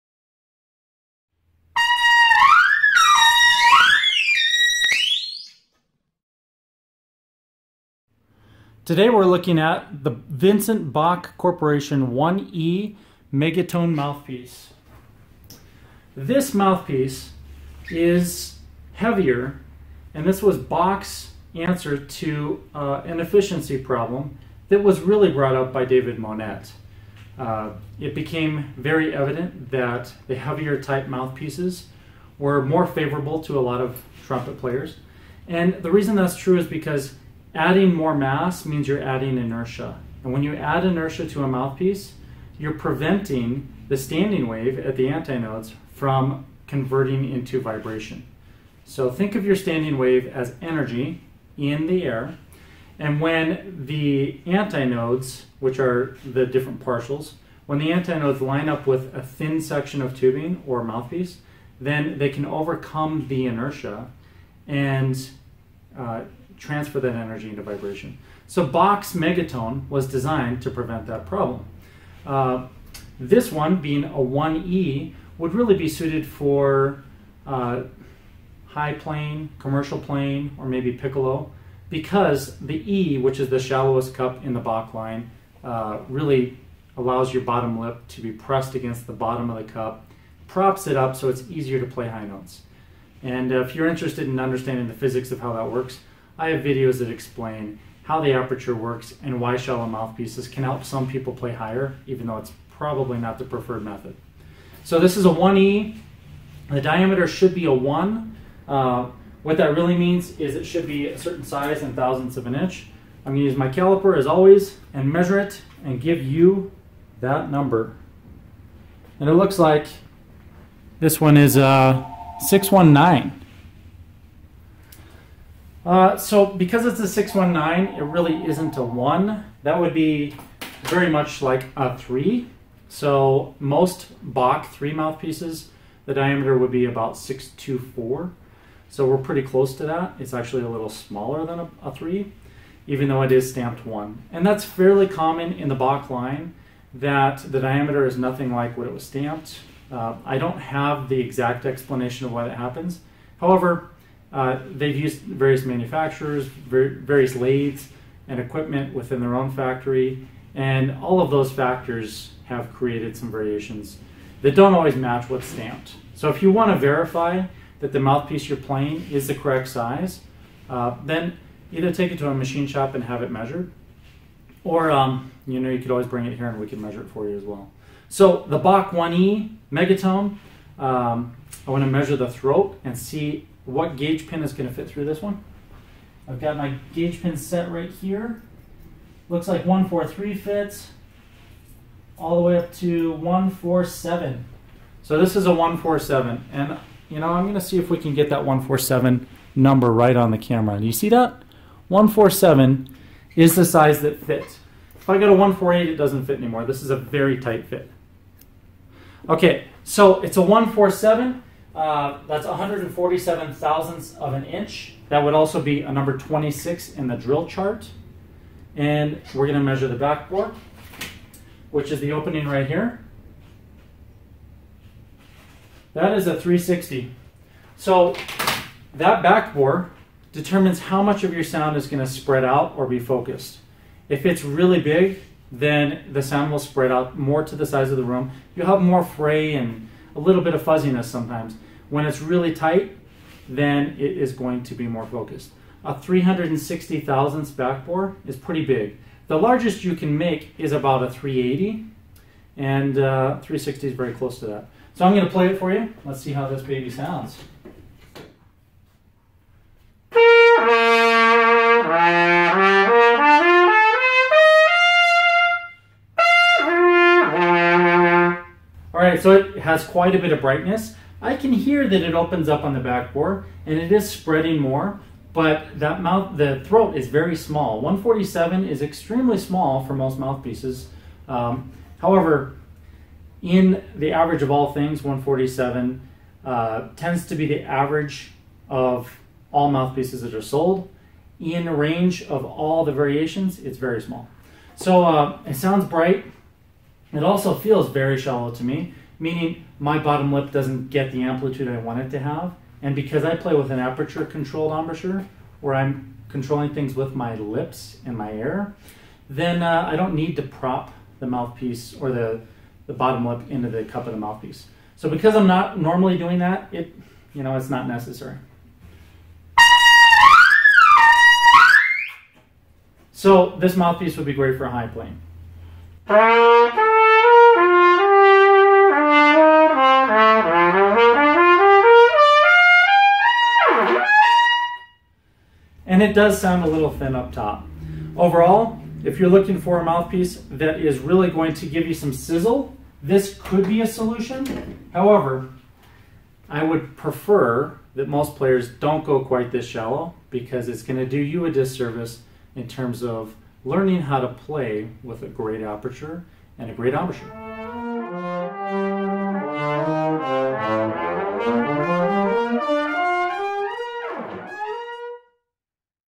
Review: Trumpet Mouthpiece - Bach 1E Megatone - piccolo trumpet or lead?
Jason reviews the Vincent Bach Corp 1E trumpet mouthpiece